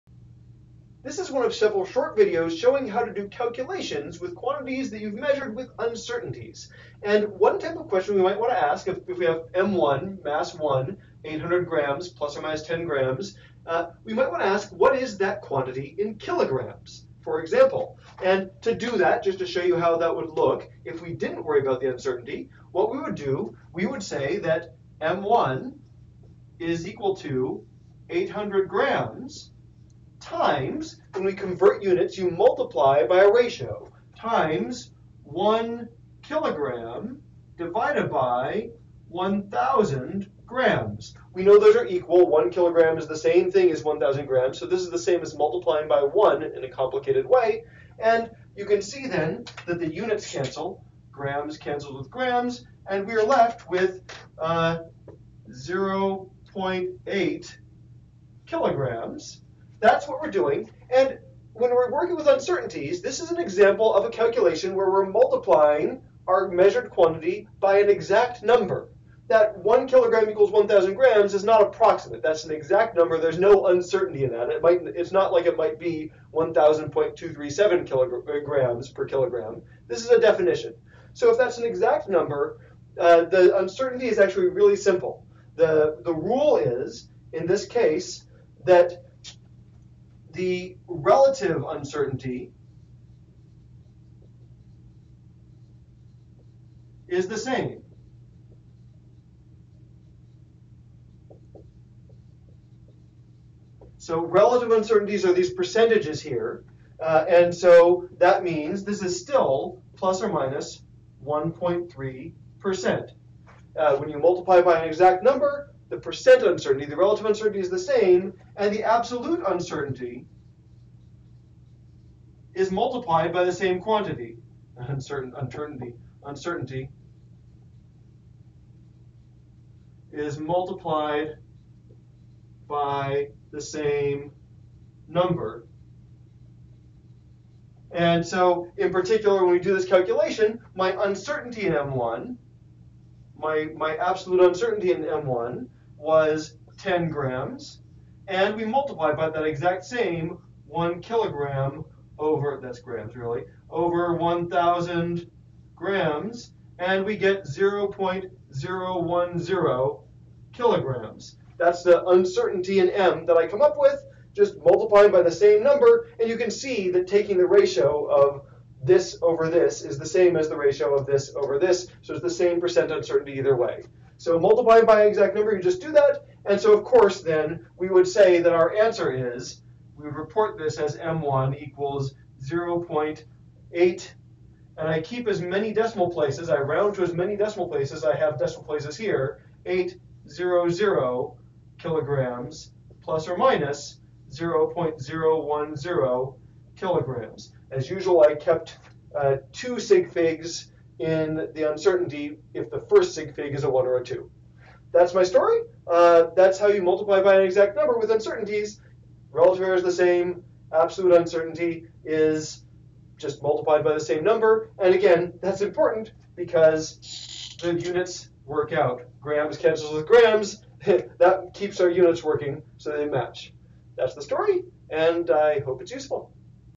Uncertainty example: Multiplying by a constant
This is one of a series of videos illustrating how to use measured data with uncertainties in calculations. In this example, we consider how to multiply by a constant (which arises here in the context of converting units).